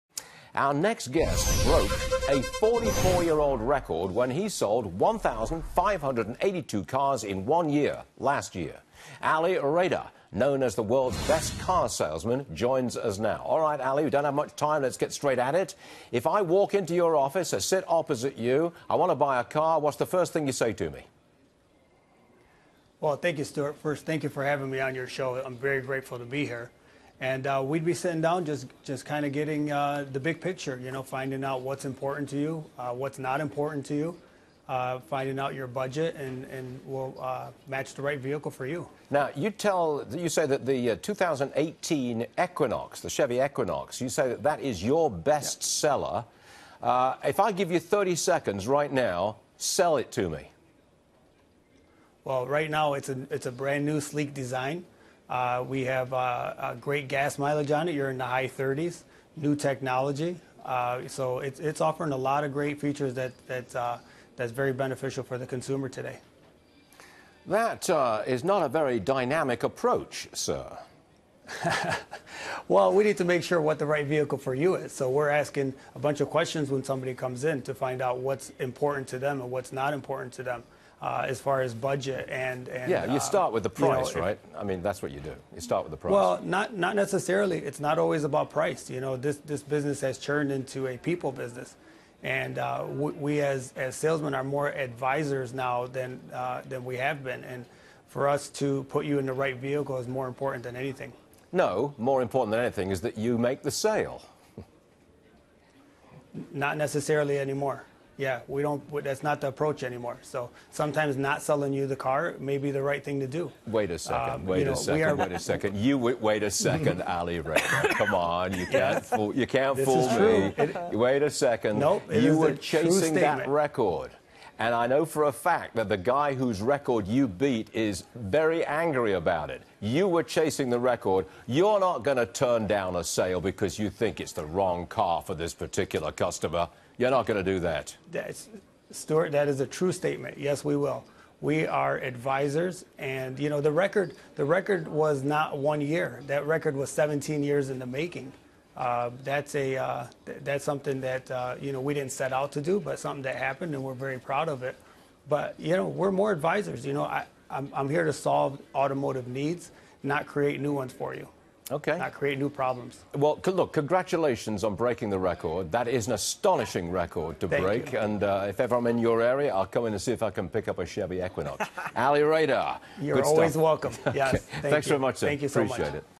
Salesman breaks record selling 1,582 cars in one year
Car salesman Ali Reda on how he broke a 44-year record for most cars sold in a year.